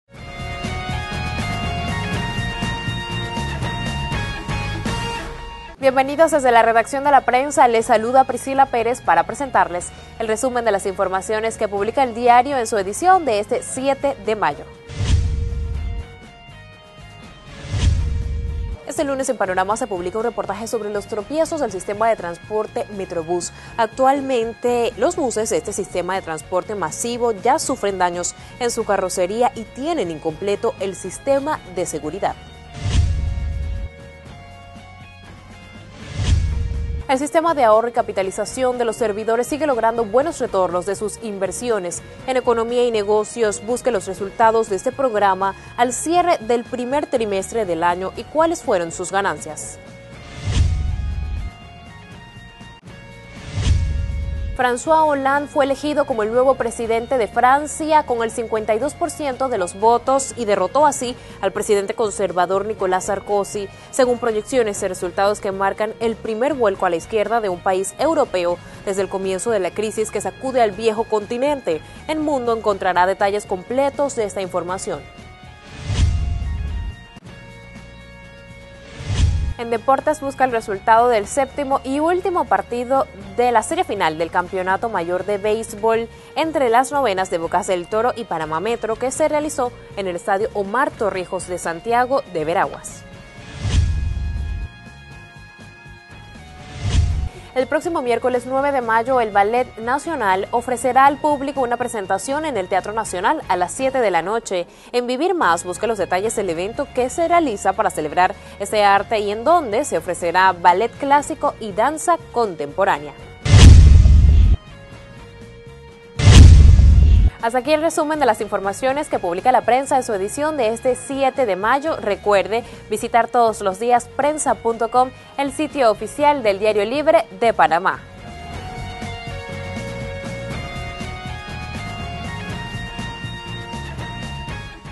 Titulares - 7 de mayo de 2012
Titulares del periódico La Prensa para la edición del 7 de mayo de 2012